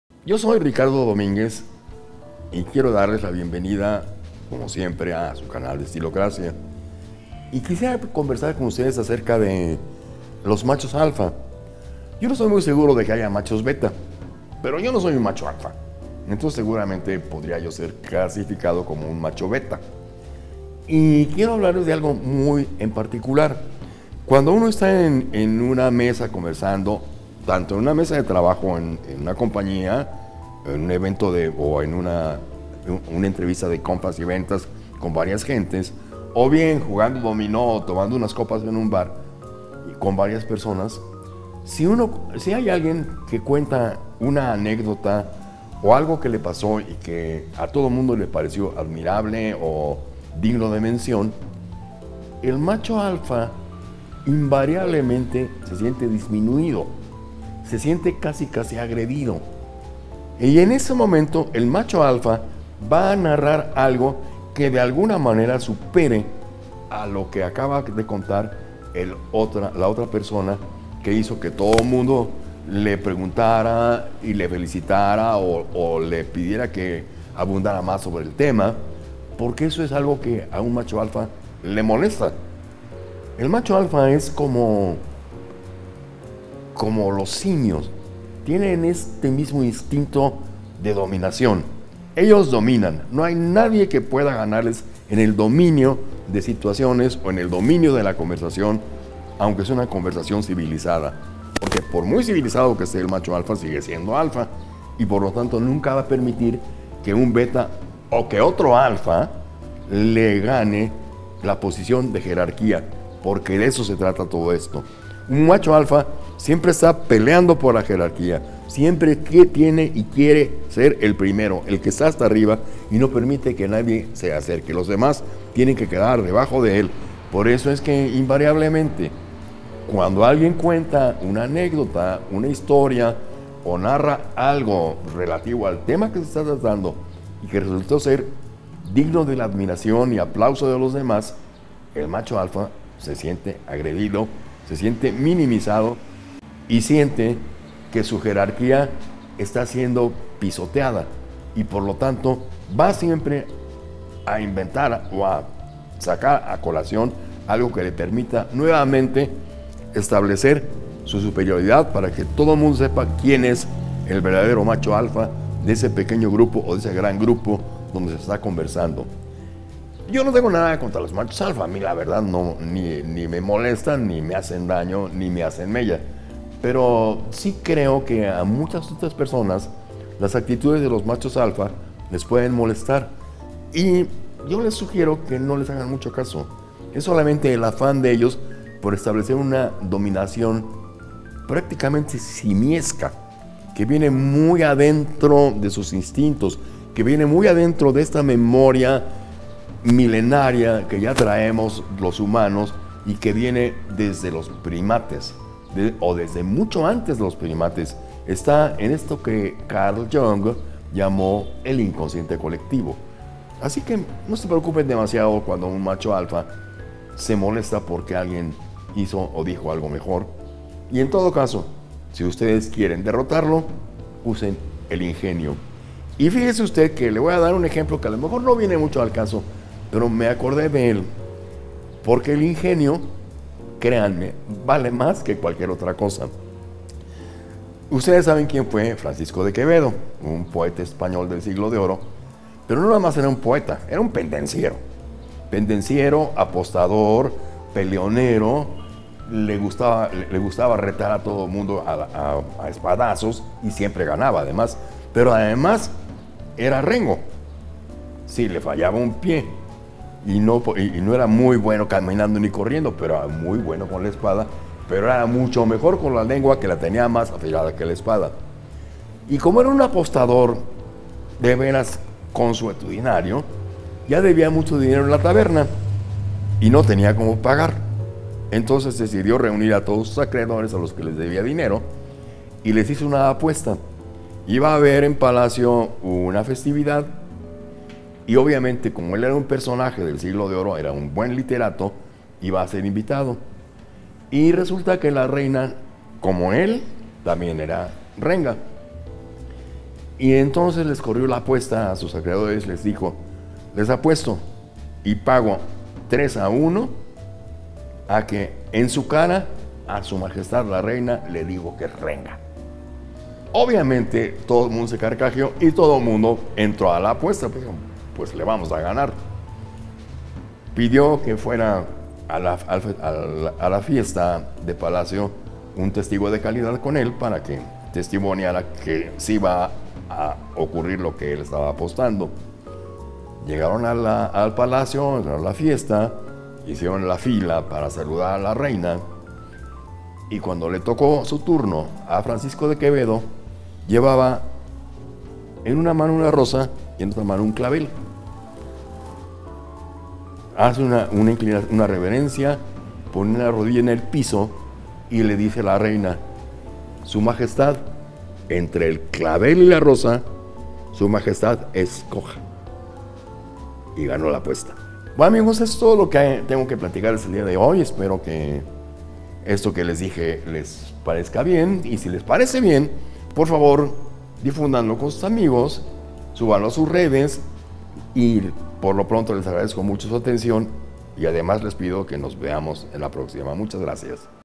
MACHO ALFA: ERRORES BÁSICOS
AÚN SI ERES UN MACHO ALFA NO TIENES TODAS LAS DE GANAR, si cometes ESTOS ERRORES CAERÁS MAL, DE VERDAD.

Visita nuestro sitio asociado de Amazon para productos de tu interés: